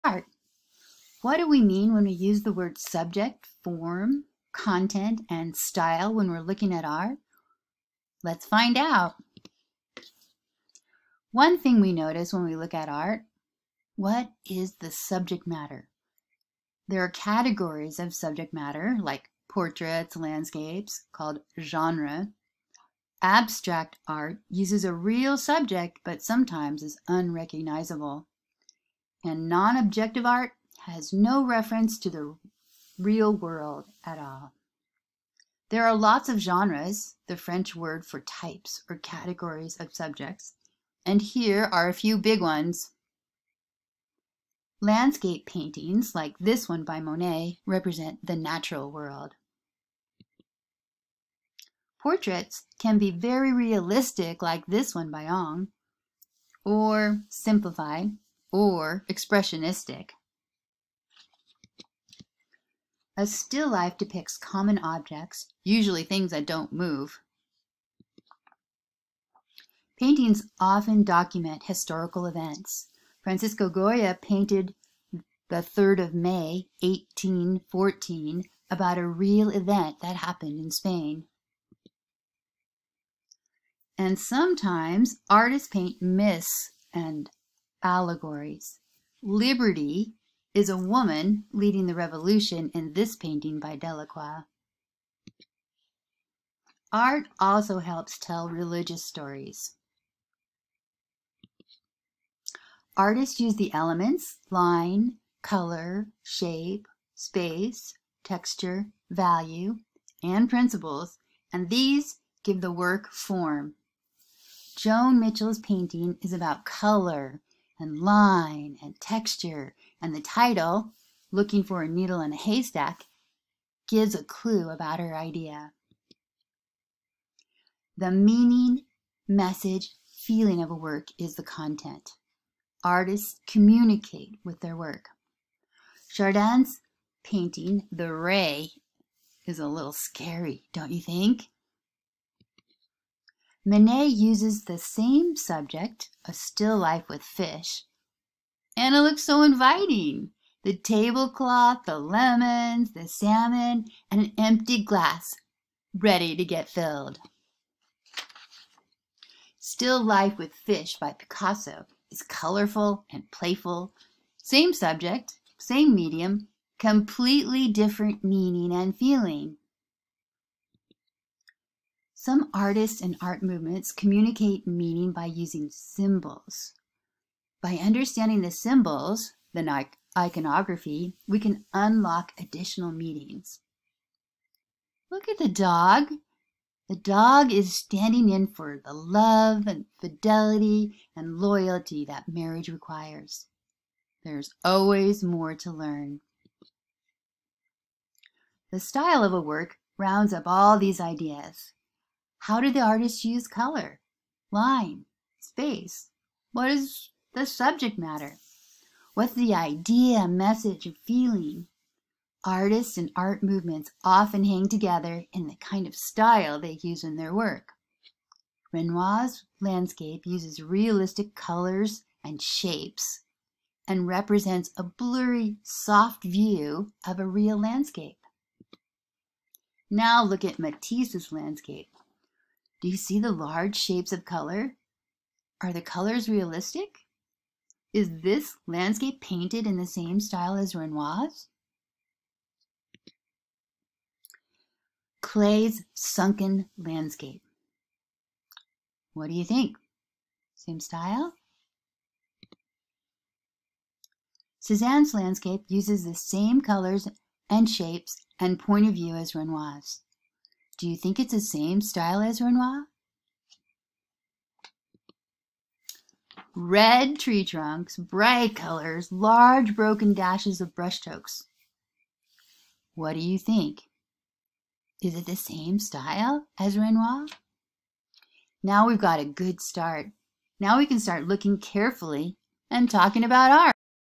looking at art: subject, form, content, style
There's lots of new vocabulary for looking at art! Learn about subject, format, content and style.